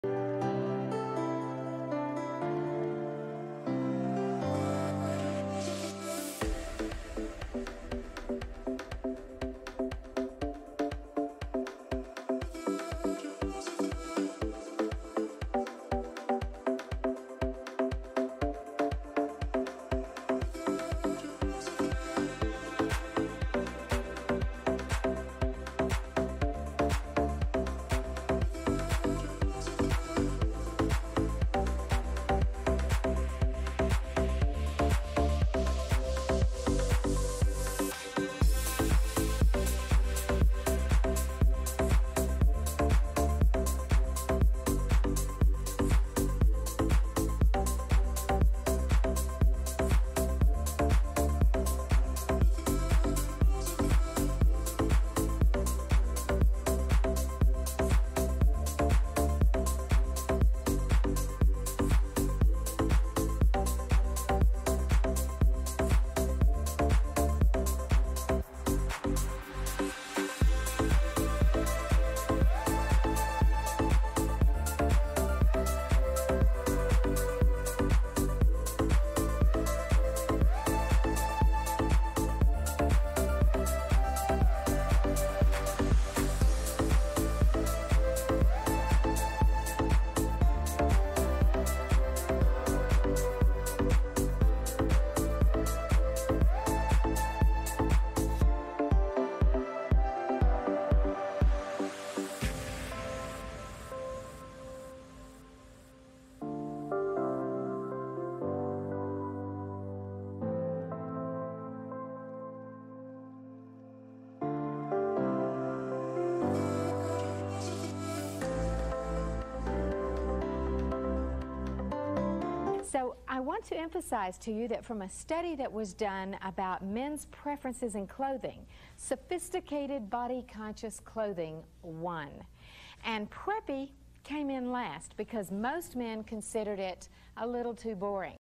Old Money Styles Rich Men hate, sorry Anna Bey 10 Ways to Look Expensive on a Budget in Spring
This video is first a visual guide to the video before it Part 1 of 7 Old Money styles rich men hate. It's also a response to @Anna Bey 's video 10 ways to look Expensive on a Budget in the Spring because it so happens that some of those styles are an example of part 1.